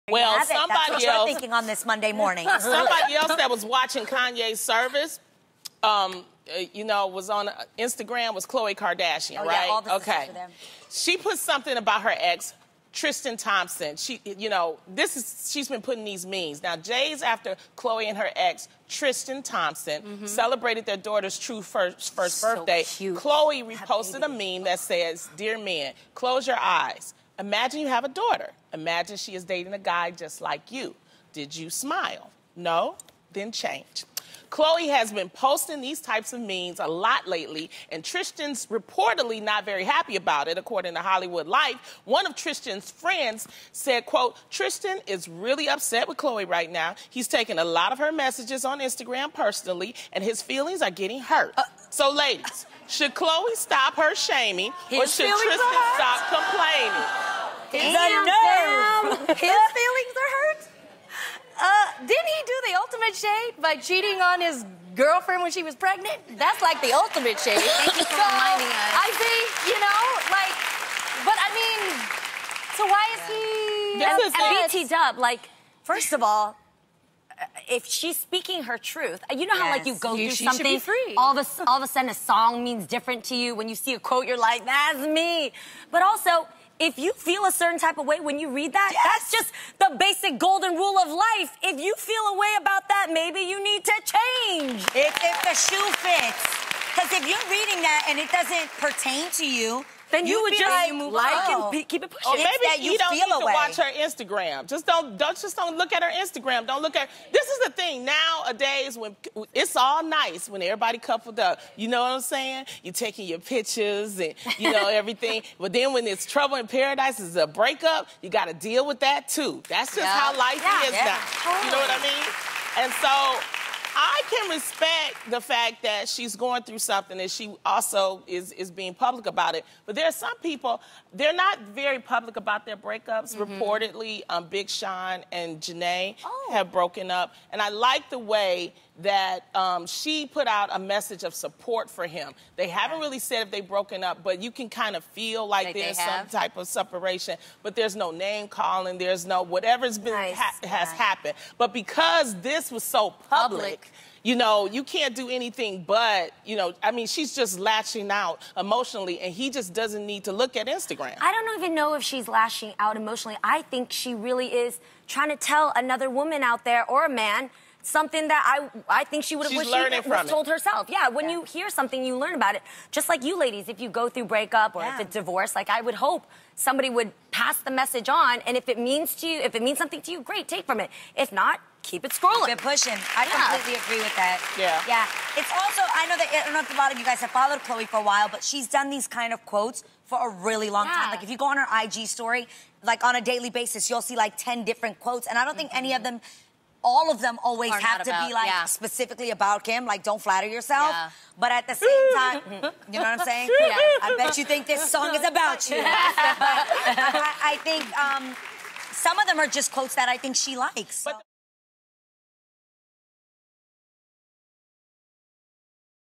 PART ONE: Why Tristan Allegedly Doesn’t Like What Khloé’s Posting on Instagram
Real fam, would you care if your ex were posting quotes that could be about your relationship?